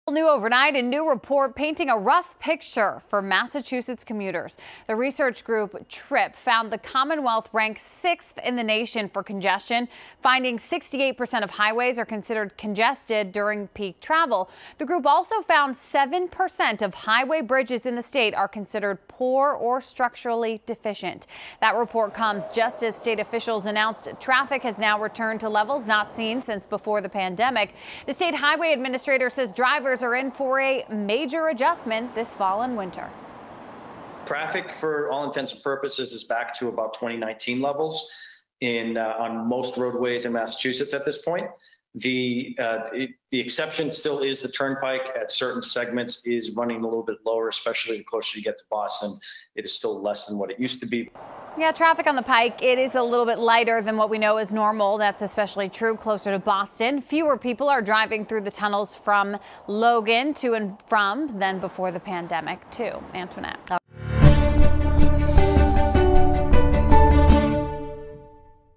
New report paints rough picture for Massachusetts commuters
Massachusetts ranks sixth in the nation fir traffic congestion, according to a new report from the research group TRIP.